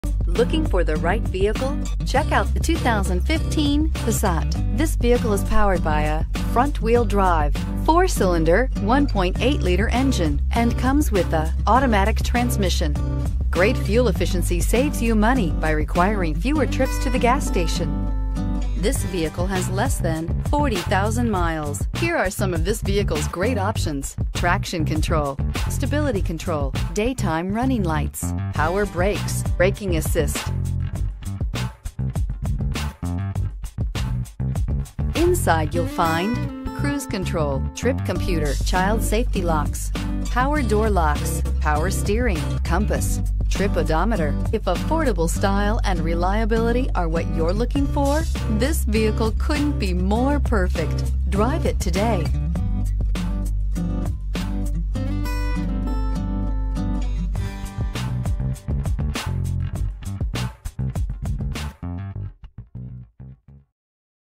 2015 Volkswagen Passat at Larry H. Miller Dodge Ram Peoria
Take home this 2015 Volkswagen Passat SE!

Pick up this Gray 2015 Volkswagen Passat, available today at Larry Miller Dodge. This could be the one you've been searching for.